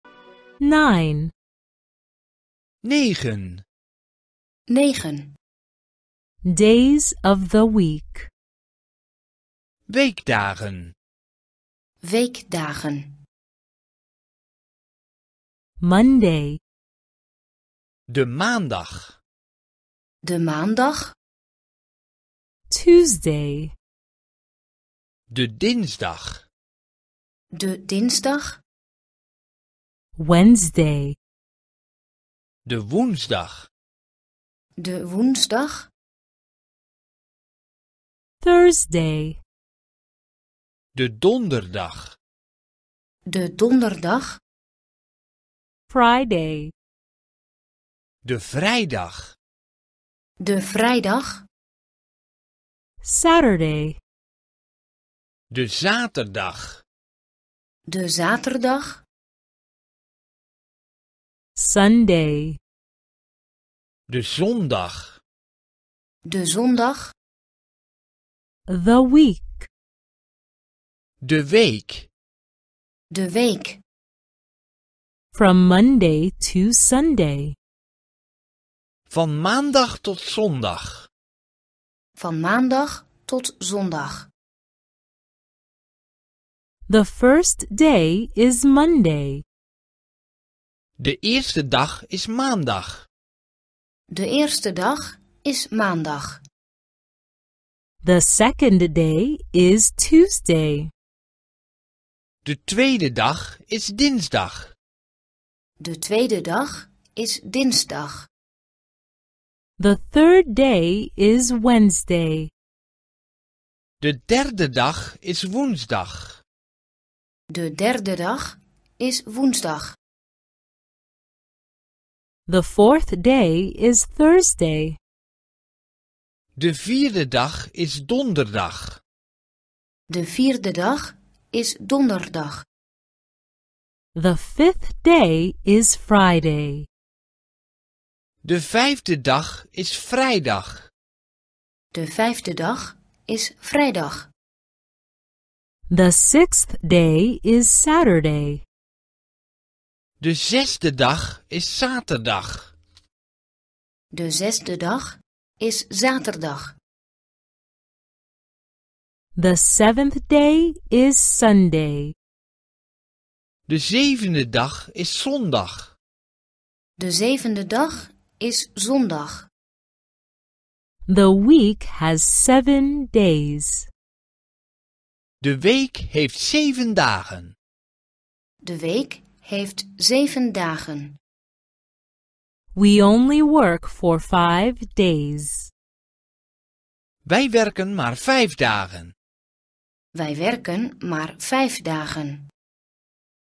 All Dutch in 100 Lessons. Learn Dutch . Most important Dutch phrases and words. Lesson 9
In this 100-lesson Dutch course, you'll learn all the important Dutch phrases and words that you need to get by in a foreign language.

If you're new to learning a foreign language, then this is the course for you! In each lesson, you'll learn a new Dutch phrase or word, and we'll provide you with a translation so you can understand the meaning. With 100 lessons, you'll be able to speak Dutch like a native in no time!